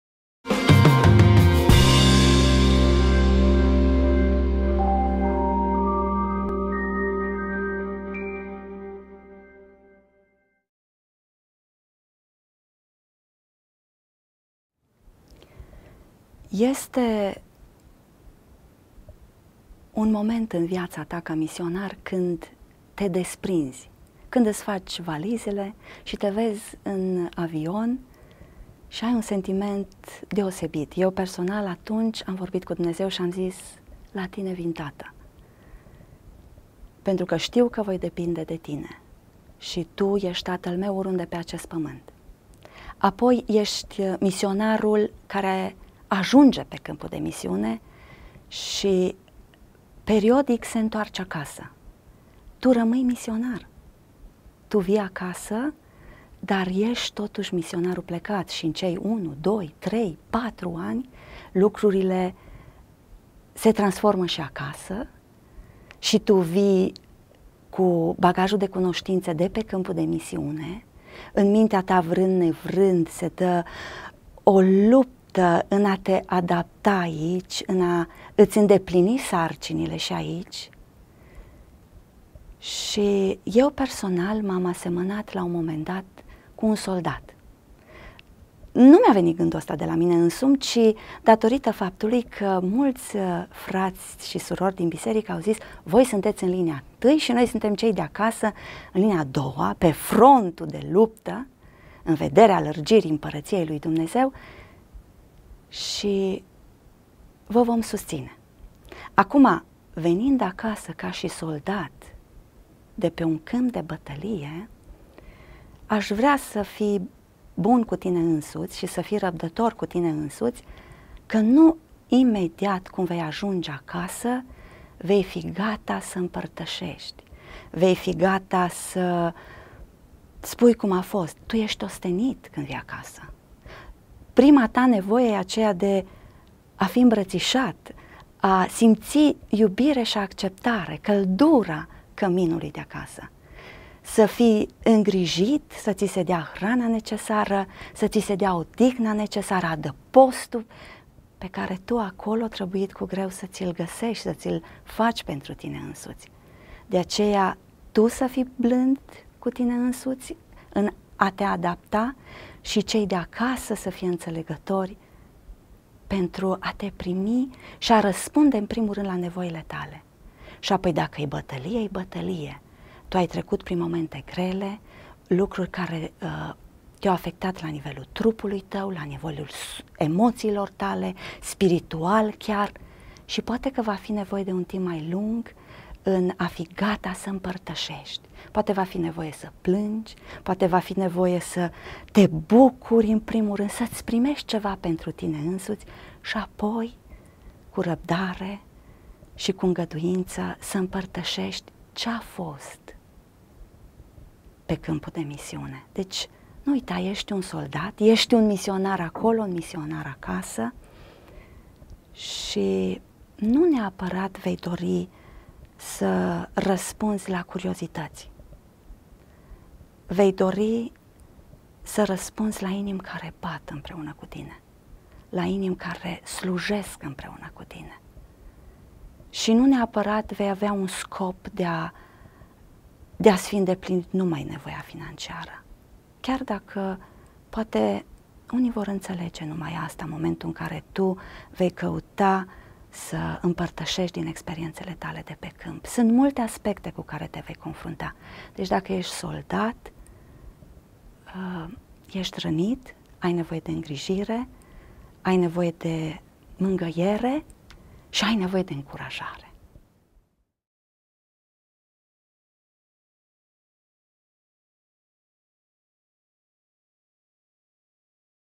Ce înseamnă să fii misionar revenit acasă?
Mirela Pașcalău, absolventă UEO și misionară, ne răspunde la întrebări.